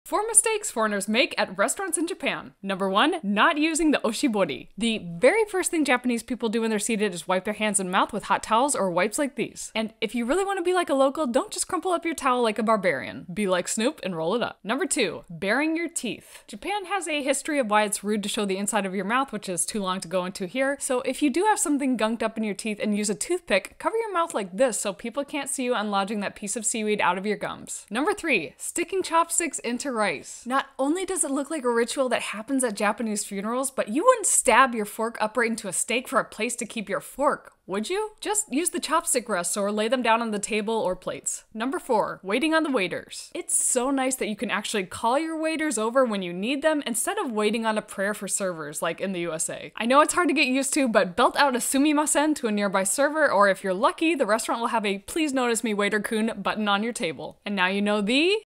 STOP Making THESE 4 Mistakes in Japan!
4 Mistakes I see tourists make at restaurants in Japan!